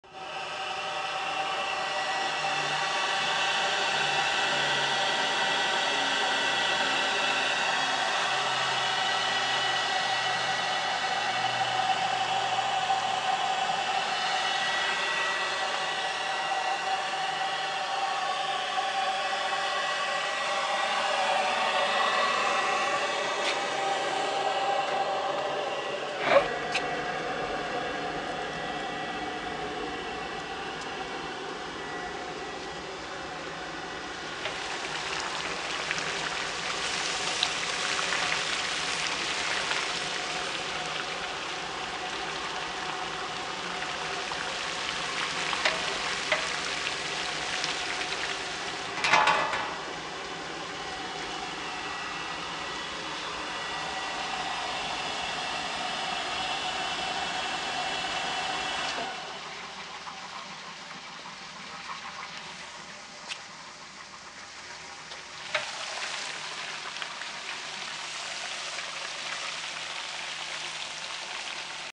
Cleveland KGL-60 gas kettle
Natural gas 60 gallon kettle. School Board kettle. In great shape.